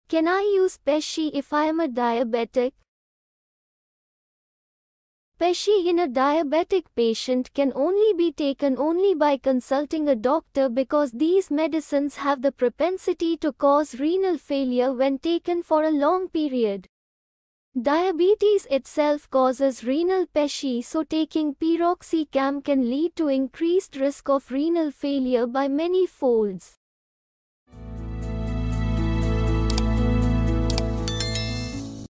Can I use Pexi if I am a diabetic?
Pexi in a diabetic patient can only be taken only by consulting a doctor because these medicines have the propensity to cause renal failure when taken for a long period. Diabetes itself causes renal Pexi so taking piroxicam can lead to increased risk of renal failure by many folds.